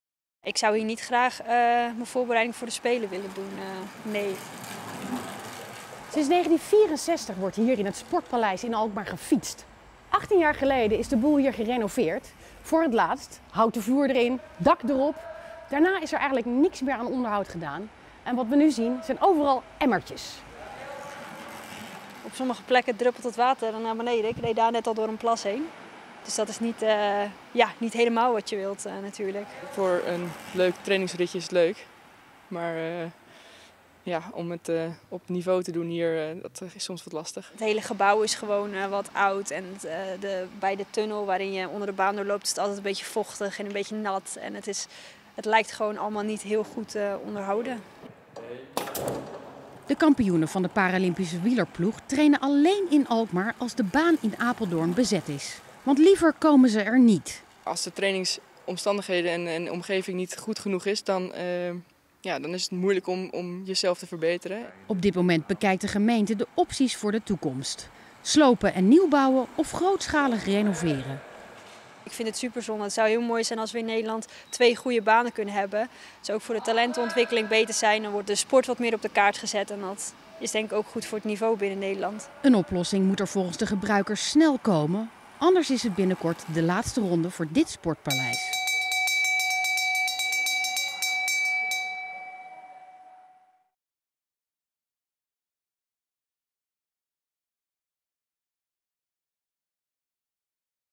Sportpaleis in deplorabele staat: "Eigenlijk fietsen we hier liever niet meer"
De baanwielrenners van TeamNL zijn blij dat ze er kunnen trainen, maar eigenlijk komen ze liever niet in het Sportpaleis. Het sportcomplex aan de Alkmaarse ring verkeert namelijk in slechte staat. Ik zou hier niet graag mijn voorbereiding voor de Spelen willen doen", zegt Caroline Groot, regerend wereldkampioen para-cycling.